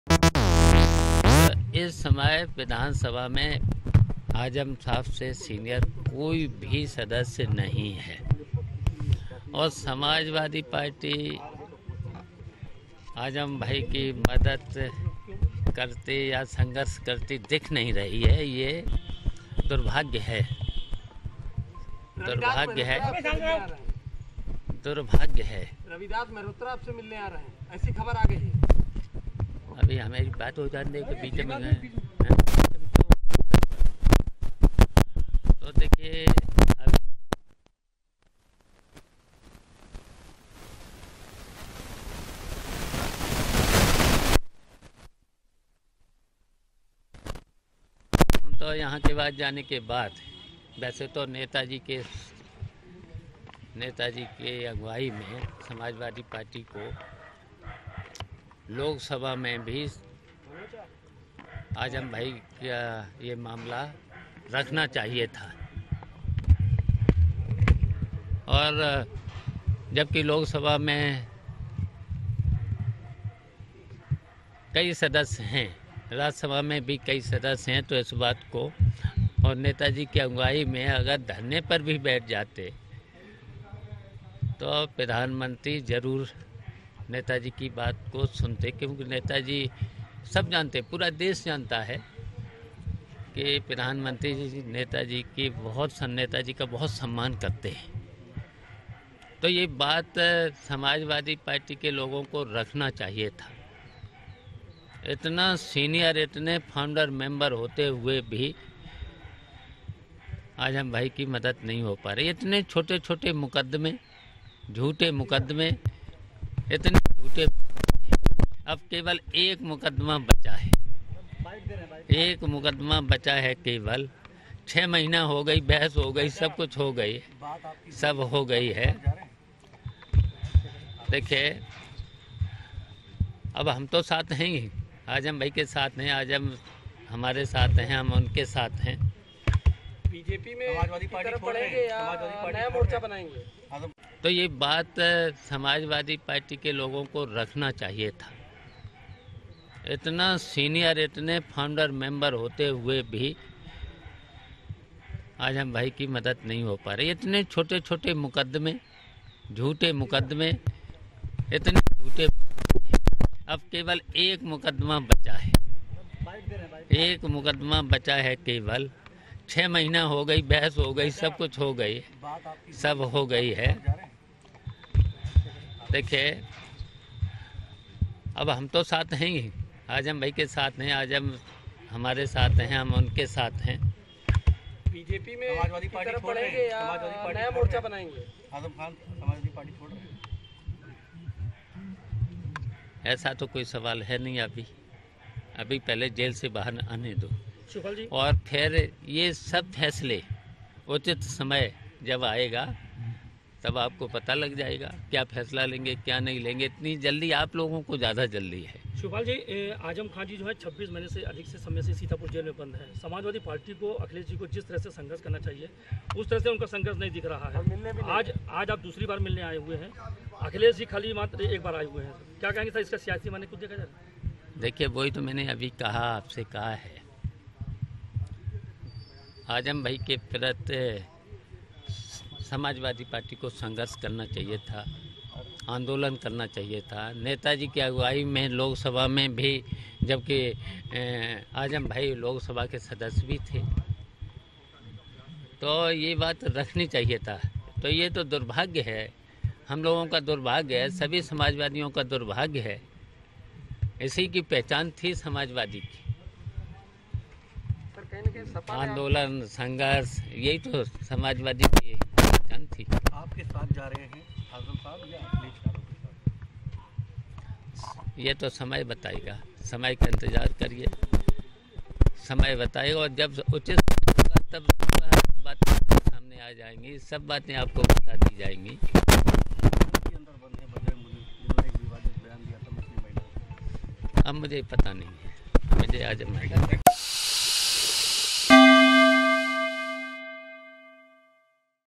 जेल से बाहर आकर Shivpal Yadav ने बताया, Azam Khan से क्या बात हुई? | Akhilesh yadav
Shivpal Singh Yadav met Azam Khan in jail today. Shivpal alleged that the Samajwadi Party was not fighting for Azam Khan. Shivpal said that Azam Khan is the senior most leader of the assembly and we should fight for him.

Edited By:Arvind